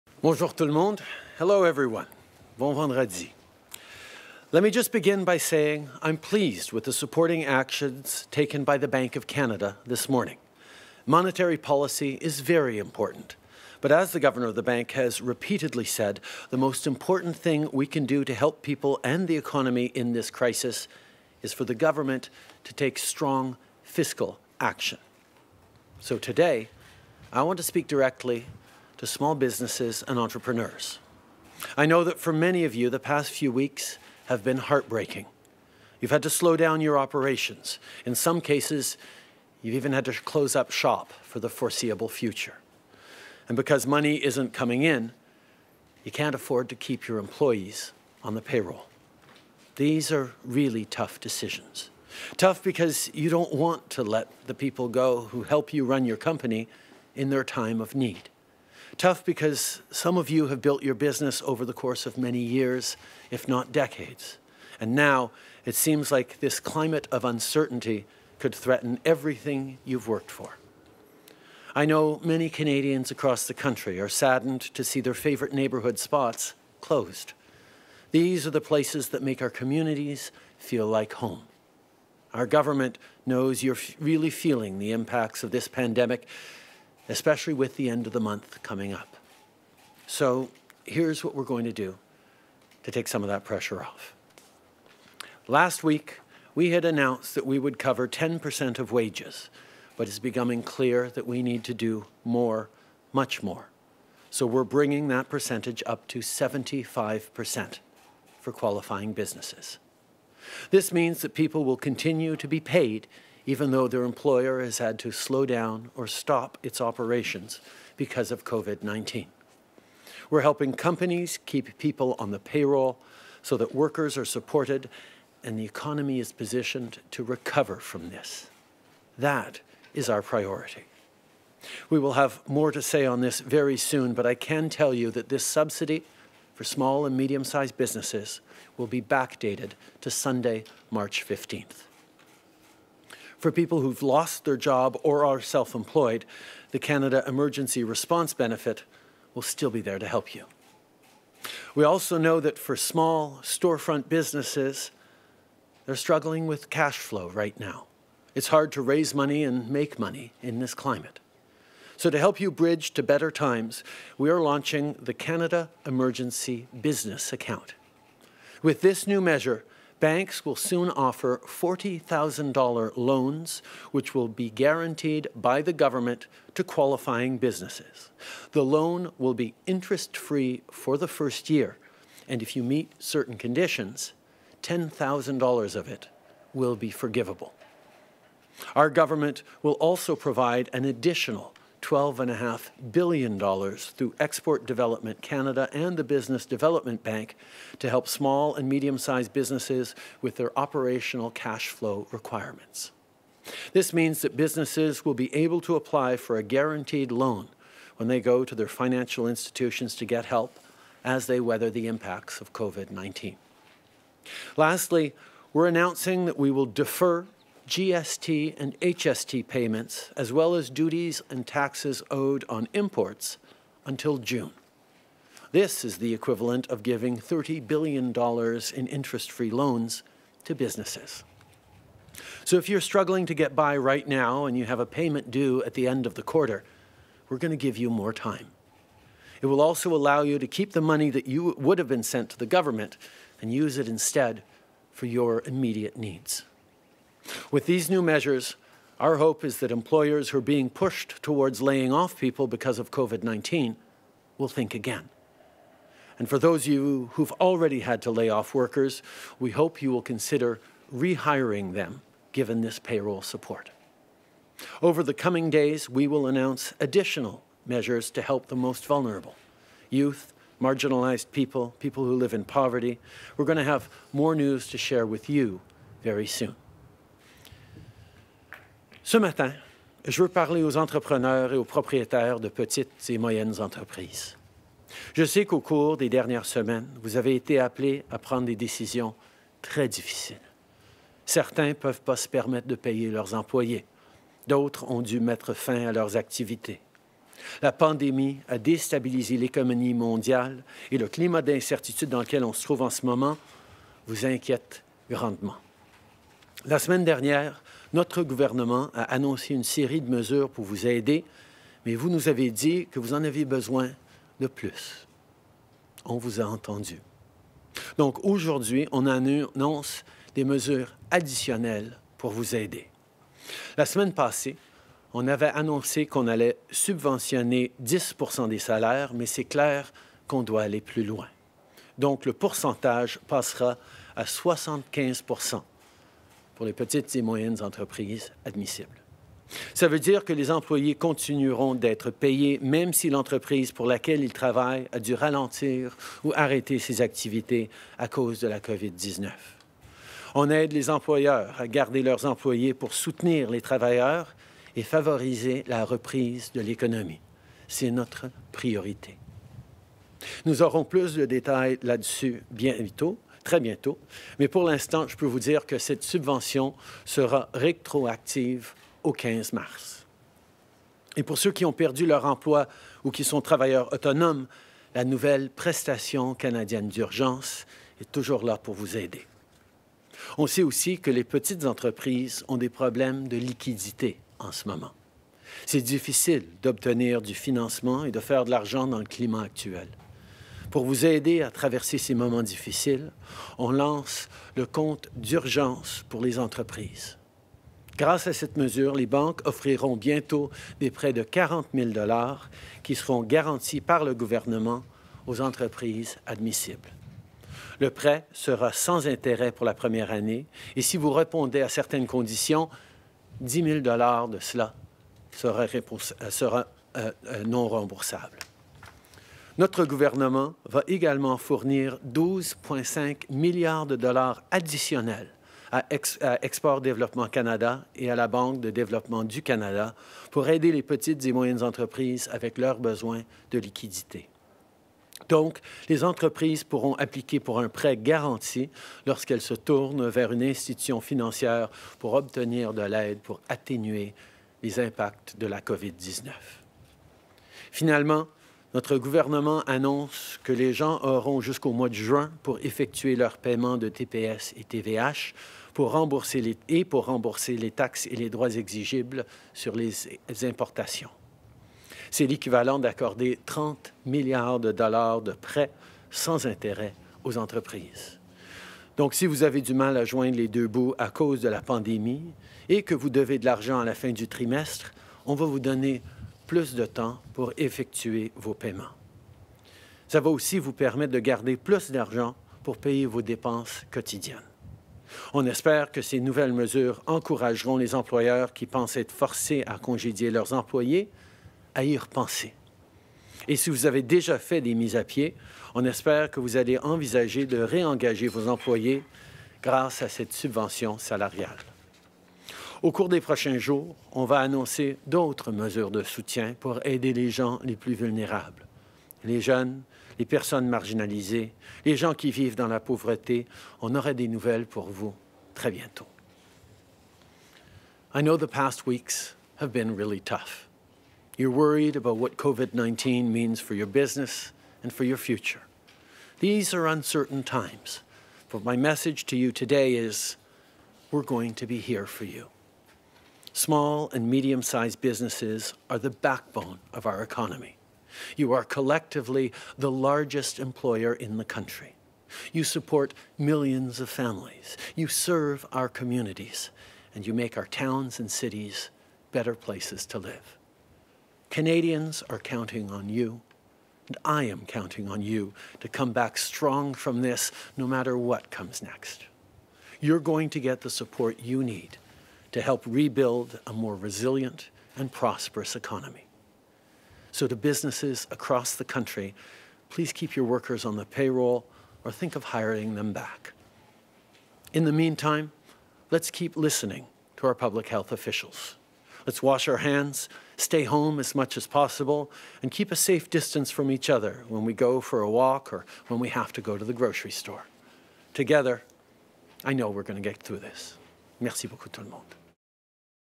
Annonce d'un soutien aux petites entreprises qui font face aux impacts de la COVID-19
27 mars 2020
Le premier ministre Justin Trudeau annonce de nouvelles mesures pour appuyer les petites entreprises qui font face aux impacts économiques de la pandémie.